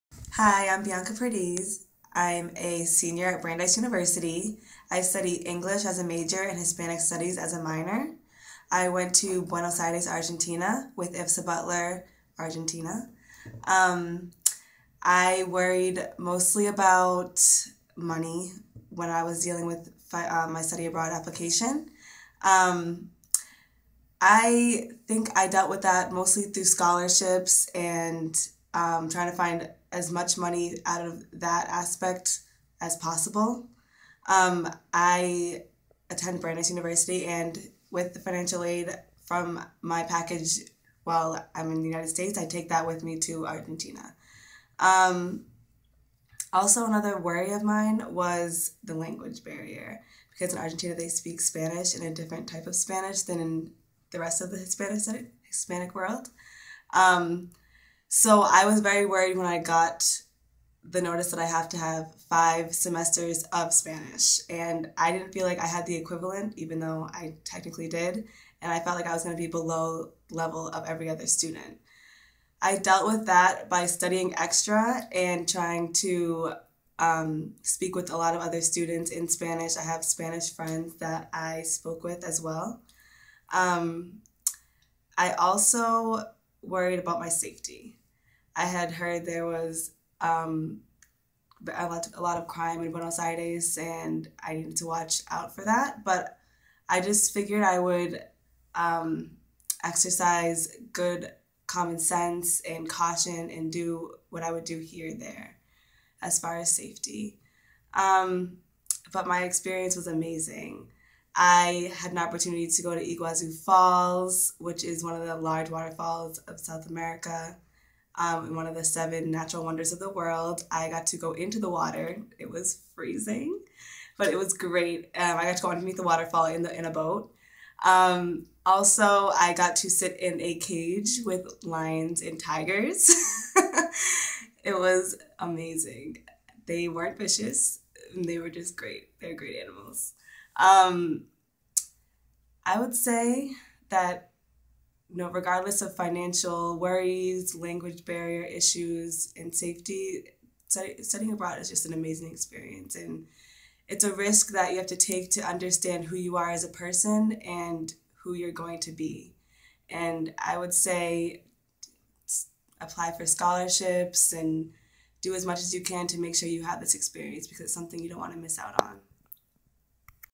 Study Abroad In Argentina | DiversityAbroad.com
Meet Bianca, an English major at Brandeis University who studied abroad in Buenos Aires, Argentina. Hear her story and learn how YOU can study abroad!